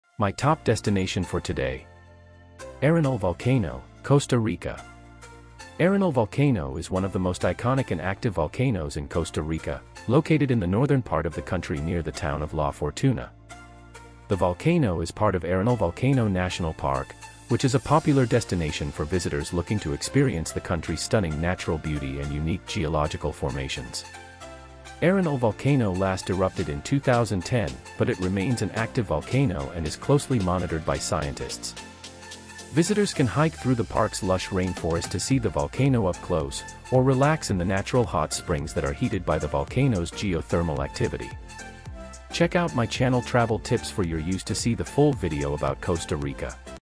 ARENAL VOLCANO, COSTA RICA !
Arenal Volcano is one of the most iconic and active volcanoes in Costa Rica, located in the northern part of the country near the town of La Fortuna. The volcano is part of Arenal Volcano National Park, which is a popular destination for visitors looking to experience the country's stunning natural beauty and unique geological formations.

Arenal Volcano last erupted in 2010, but it remains an active volcano and is closely monitored by scientists. Visitors can hike through the park's lush rainforest to see the volcano up close, or relax in the natural hot springs that are heated by the volcano's geothermal activity. The park also offers a variety of outdoor activities, including zip lining, horseback riding, and rafting.

Arenal Volcano and the surrounding area are known for their stunning landscapes, including the nearby Lake Arenal and the Arenal Hanging Bridges, a series of suspension bridges that allow visitors to walk through the rainforest canopy. The area is also home to a wide range of wildlife, including monkeys, toucans, and sloths.